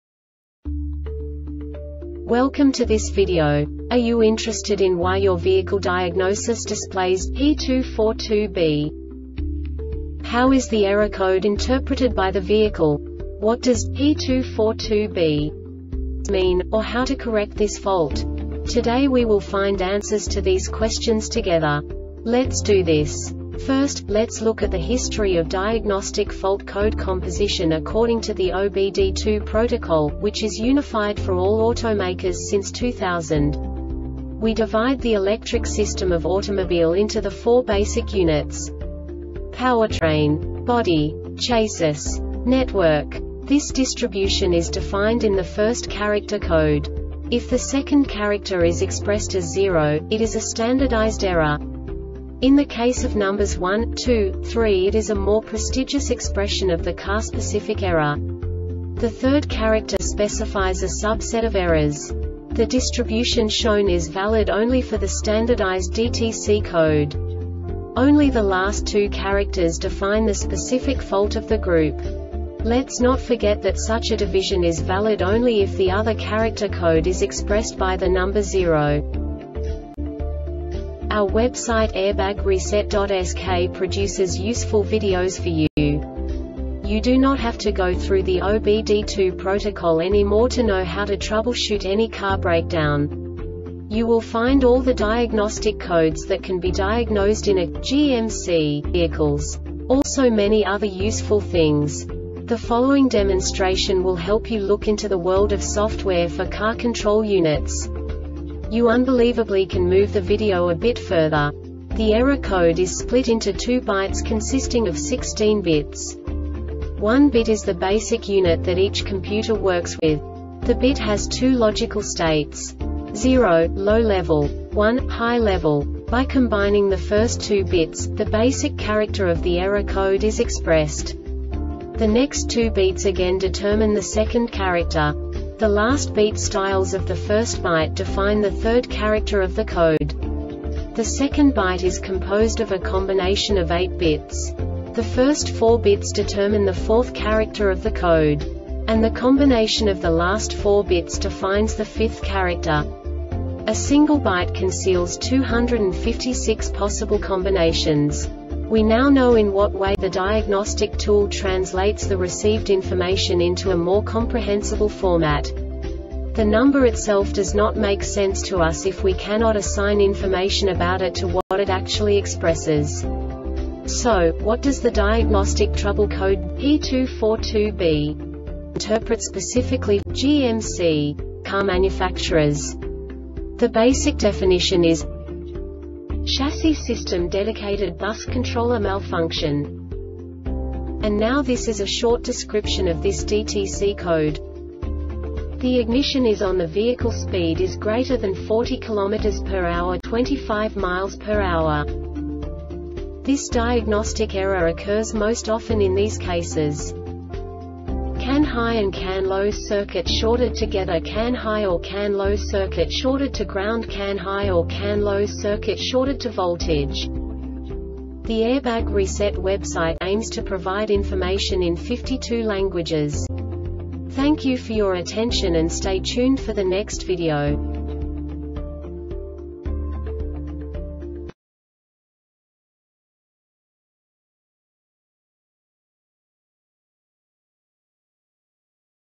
DTC GMC P242B Short Explanation
Contents:
0:21 Basic DTC analysis according to OBD2 protocol standard.
1:48 Insight into programming
2:58 A diagnostically understandable description of the Diagnostic Trouble Code
3:05 Basic error definition

Make:
GMC
Code:
P242B
Definition:
Chassis System Dedicated Bus Controller Malfunction
Description:
The ignition is ON.The vehicle speed is greater than 40 km or h (25 mph).
Cause:
CAN HI and CAN LO circuits shorted together.CAN HI or CAN LO circuit shorted to ground.CAN HI or CAN LO circuit shorted to voltage.